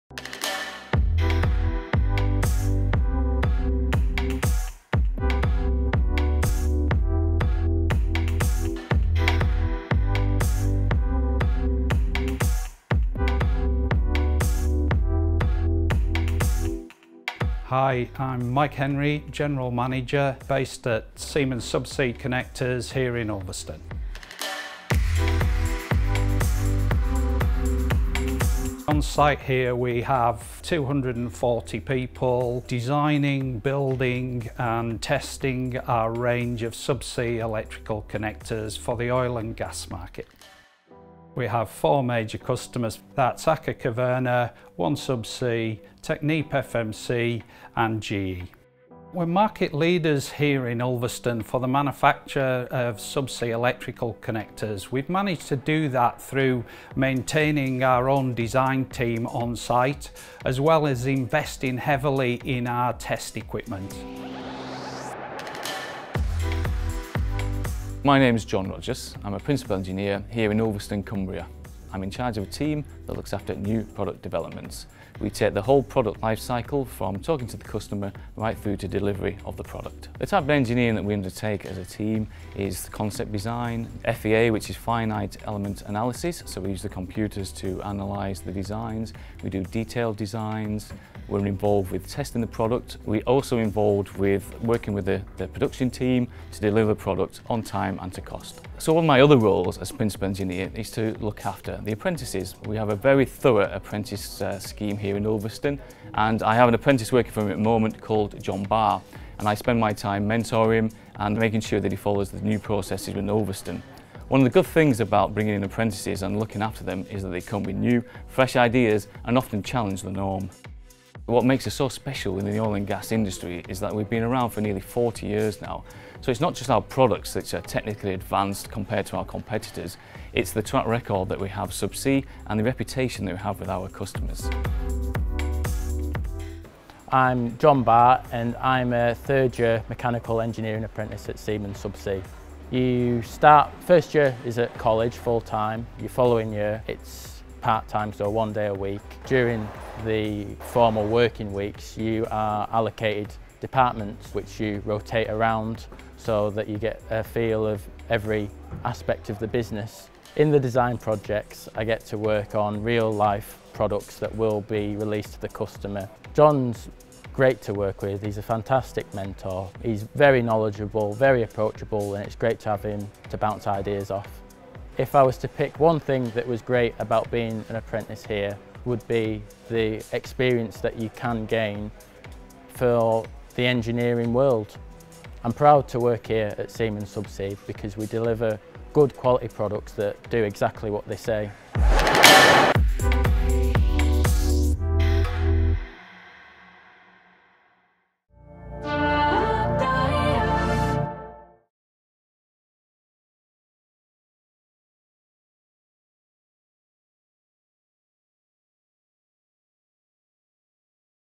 Siemens Subsea (Ulverston)
Take a closer look at some of our businesses and the people behind them.  This 3.5 min video features the Subsea business in Ulverston, Cumbria (UK). Hear General Manager Mike Henry talk about what the business does and who their customers are, find out about New Product Development from John Rogers, and John Barr tells us what the best thing is about being a Siemens apprentice.